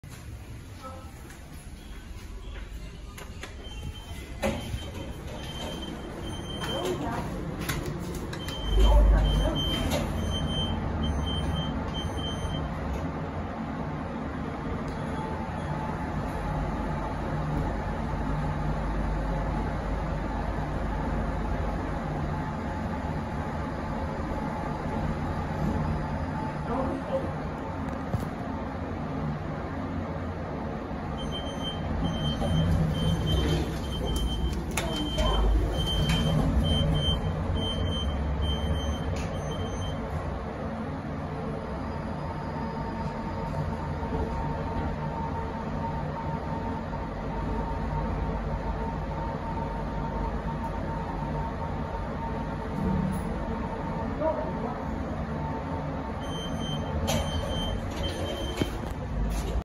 HDB Block 151 Mei Ling (Queenstown) || Fujitec Elevator (Lift B)
Recorded on 26 June 2023. [VIDEO #3046] This is Block 151 Mei Ling Street, a confined-interior corridor block in Queenstown, Singapore. There are 10 storeys in this block, also 3 lifts in this block. All are Fujitec. Lift B is serving 8 storeys.

Specifications:
Floors served: 1 - 8
Manufacturer: Fujitec
Year Installed: 1999
Notes: Replaced 1968 Hitachi lift